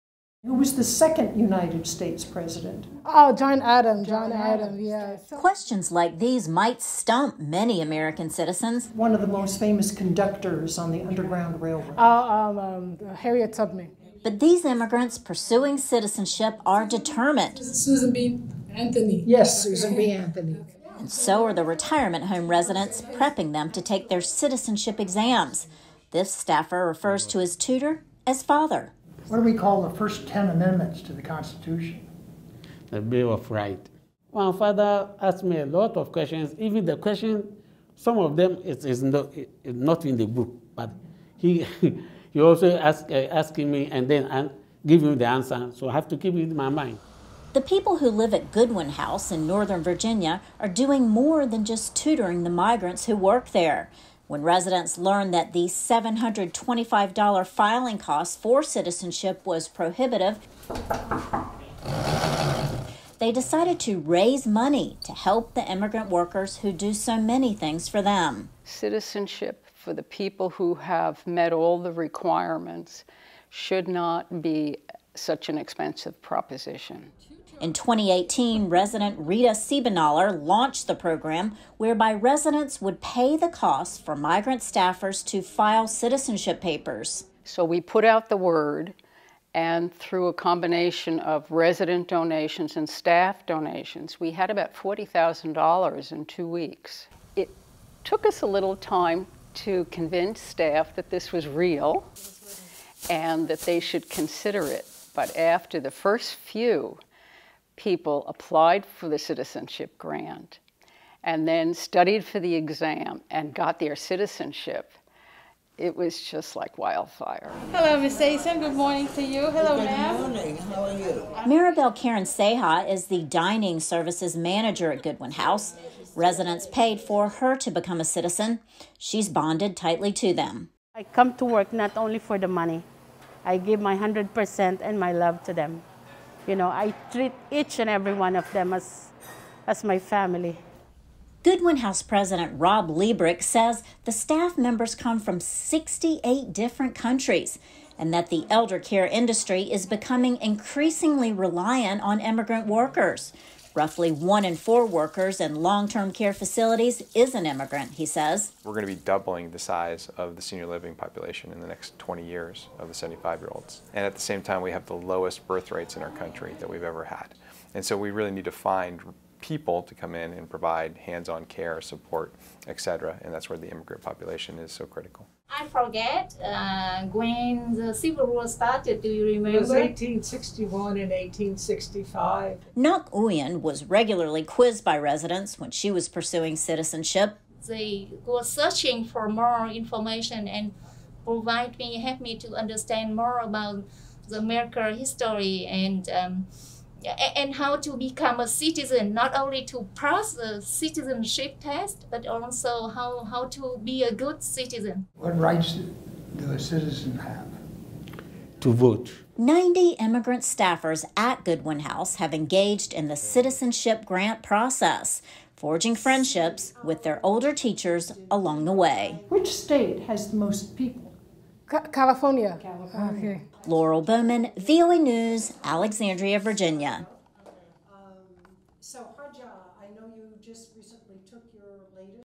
Retirees at Virginia Senior Homes Help Immigrant Staff Gain US Citizenship
The immigrant workers at the Goodwin House retirement homes in Northern Virginia come from 68 different countries. When residents learned that the $725 fee to file for citizenship was a deterrent for many staff, they decided to help the immigrant workers who help them. VOA’s Laurel Bowman has more.